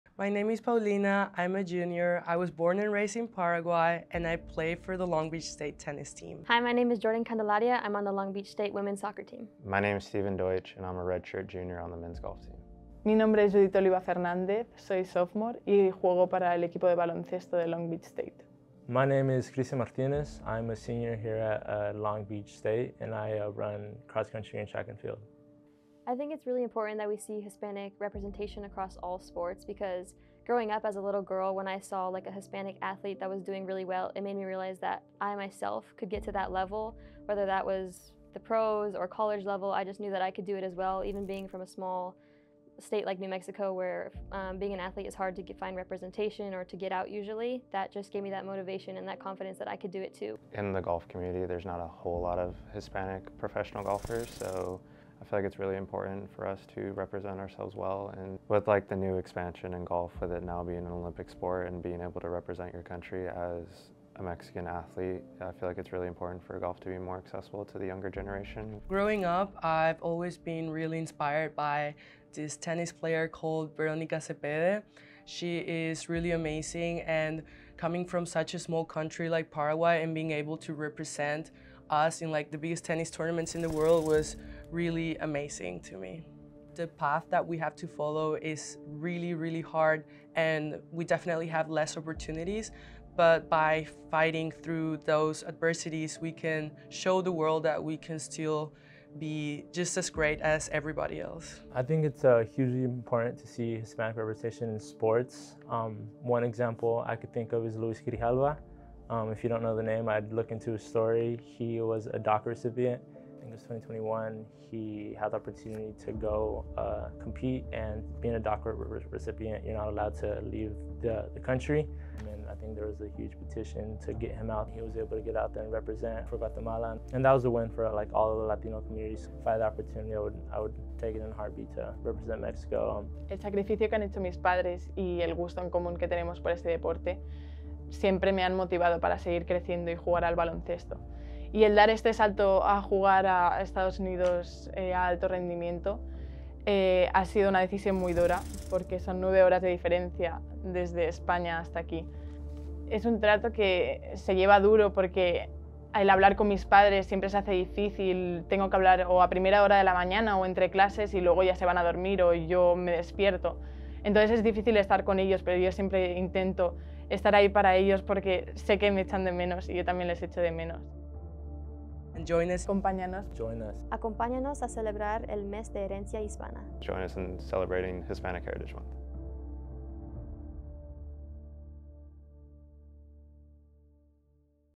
Long Beach State Athletics Celebrates Hispanic Heritage Month
Join us in celebrating Hispanic Heritage Month, as we highlight our student athletes who represent the beauty of the Hispanic experience.